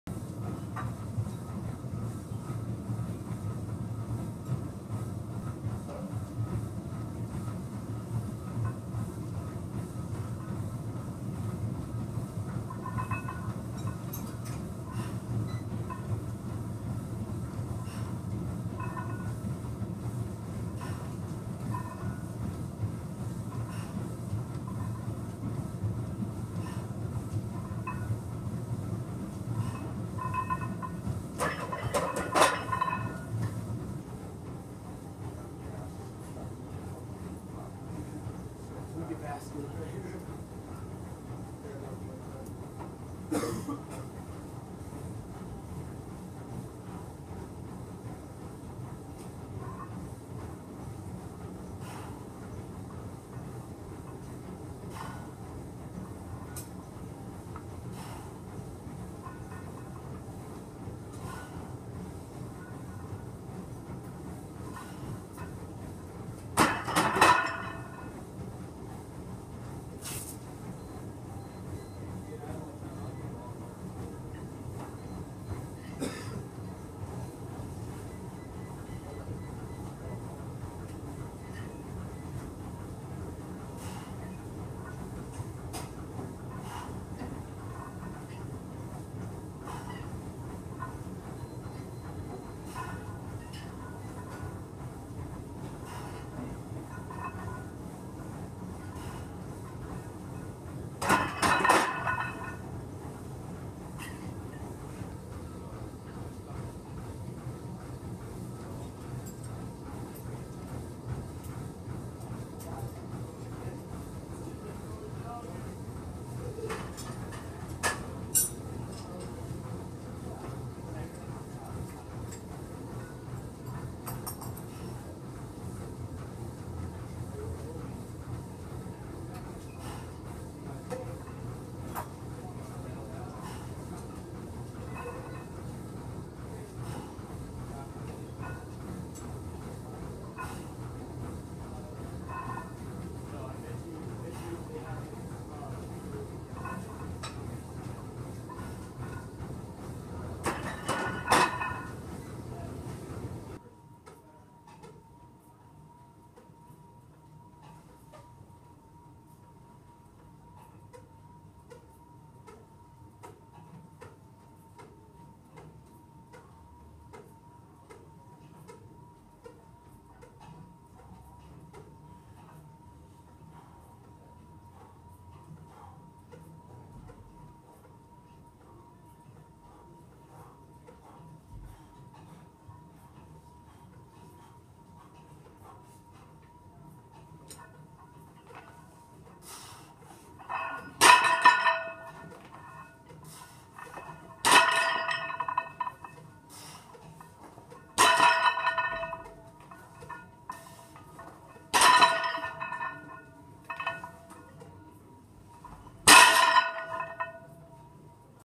MS Legs 1/2/15 HB Squat 340, Deadlift 505
High Bar Squats 340x4x6
Deadlifts (straps) 505x5

Need to work on High bar stance and depth.
Grip very weak from tendinitis. Deadlift form was ruff, need to deadlift more.